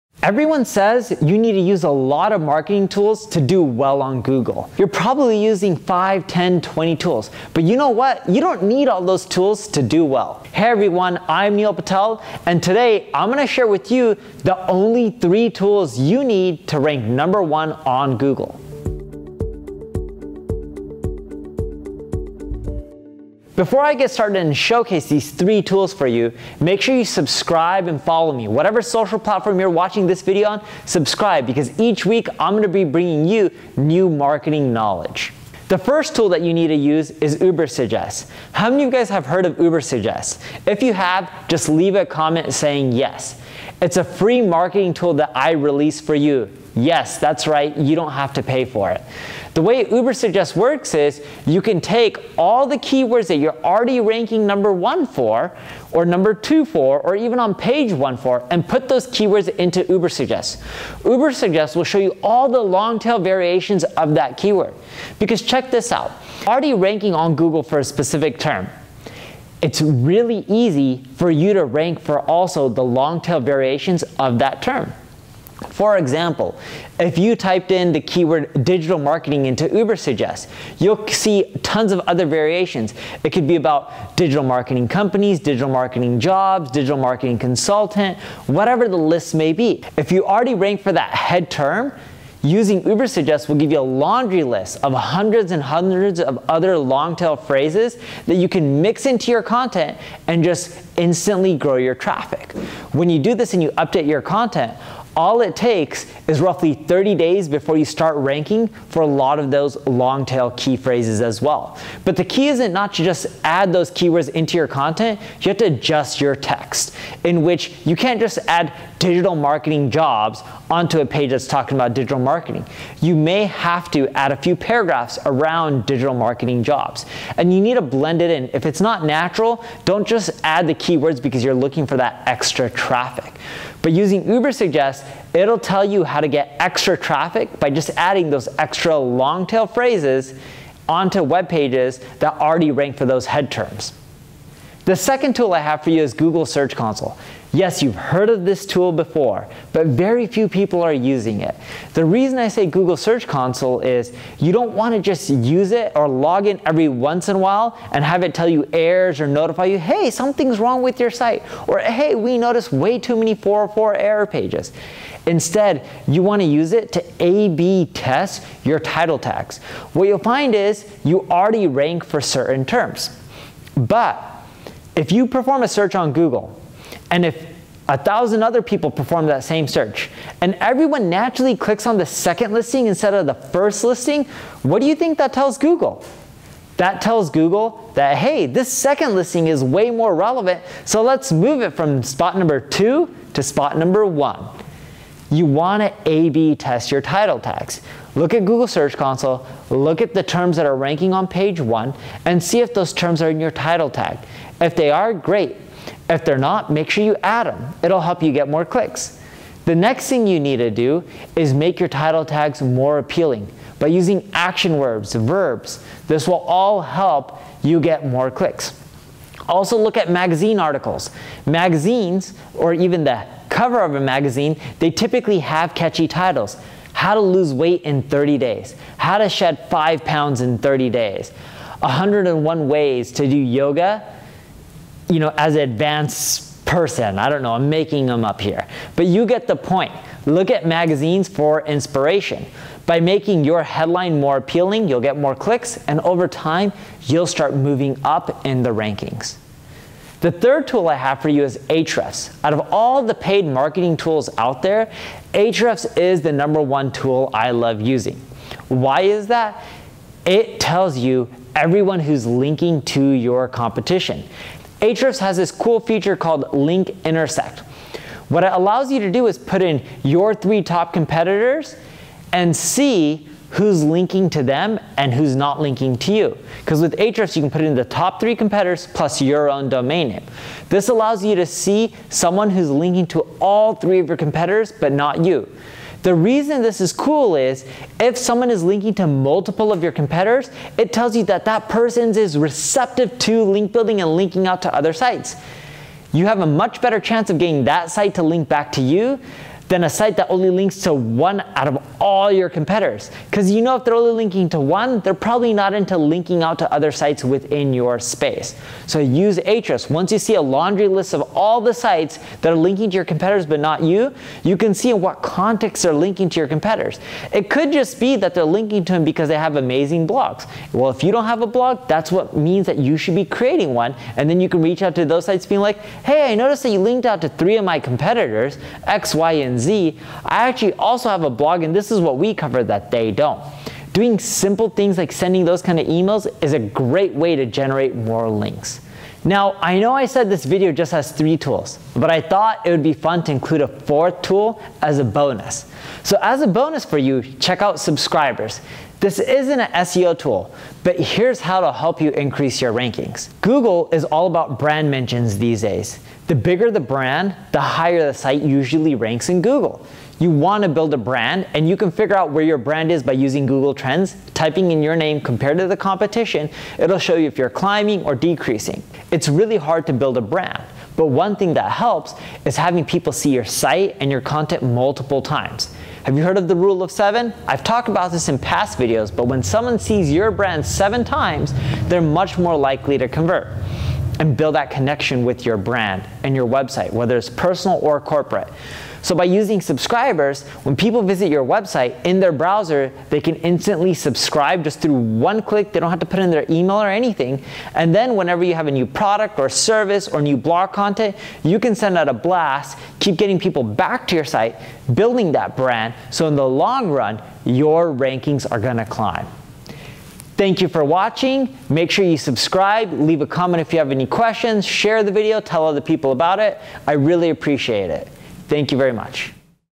The 3 SEO Tools I Use Rank #1 on Google | Neil Patel
Today I'm going to share with you the only three SEO tools you need to rank number one on Google. Everyone says you need to use a lot of marketing tools to do well on Google. You're probably using five, ten, 20 tools. But you know what? You don't need all those SEO tools to do well. In this video you’ll learn about the only 3 SEO tools that will help you rank #1 on Google.

TRANSCRIPTION NOTES:
The first tool you need to use is Ubersuggest. How many of you guys have heard of Ubersuggest? If you have, just leave a comment saying yes. It's a free marketing tool that I release for you. Yes, that's right, you don't have to pay for it. The way Ubersuggest works is you can take all the key words that you're already ranking number one for, or number two for, or even on page one for, and put those key words into Ubersuggest. Ubersuggest will show you all the long-tail variations of that key word.

The second tool I have for you is Google Search Console. Yes, you've heard of this tool before. But very few people are using it. The reason I say Google Search Console is you don't want to just use it or log-in every once in awhile and have it tell you errors or notify you "Hey, something's wrong with your site." Or, "Hey, we notice way too many 404 error pages." Instead, you wanna use it to A/B Test Your Title Tags. What you'll find is, you already rank for certain terms. But, if you perform a search on Google, and if a 1000 other people perform that same search, and everyone naturally clicks on the second listing instead of the first listing, what do you think that tells Google? That tells Google that hey, this second listing is way more relevant, so let's move it from spot number two to spot number one. You wanna A/B Test Your Title Tags. Look at Google Search Console, look at the terms that are ranking on page one and see if those terms are in your title tag. If they are, great. If they're not, make sure you add 'em. It'll help you get more clicks.

The third tool I have for you is ahrefs. Out of all the paid marketing tools out there, ahrefs is the number one tool I love using. Why is that? It tells you everyone who is linking to your competition. ahrefs has this cool feature called link intersect. What it allows you to do is put in your three top competitors and see who's linking to them and who's not linking to you. Cause with ahrefs, you can put in the top three competitors plus your own domain name. This allows you to see someone who's linking to all three of your competitors, but not you. The reason this is cool is, if someone is linking to multiple of your competitors, it tells you that that persons is receptive to link building and linking out to other sites. You have a much better chance of getting that site to link back to you than a site that only links to one out of all your competitors. Cause you know if they're only linking to one, they're probably not into linking out to other sites within your space. So use ahrefs. Once you see a laundry list of all the sites that are linking to your competitors but not you, you can see what contacts are linking to your competitors. It could just be they're linking to them because they have amazing blogs.